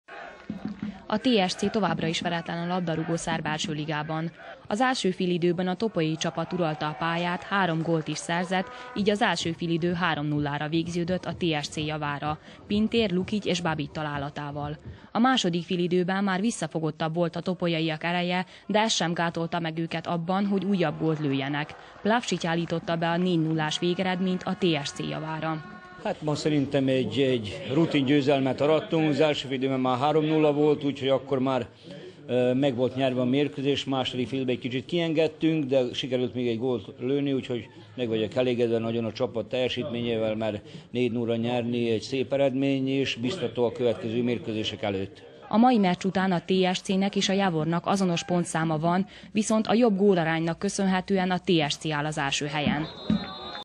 A TSC a táblázat élén
Sikerült a TSC-nek a bravúr a labdarúgó szerb első liga 25. fordulójában. A topolyaiak a zentai stadionban 4:0-ra győztek az užicei Sloboda ellen, és ezzel a táblázat első helyére kerültek.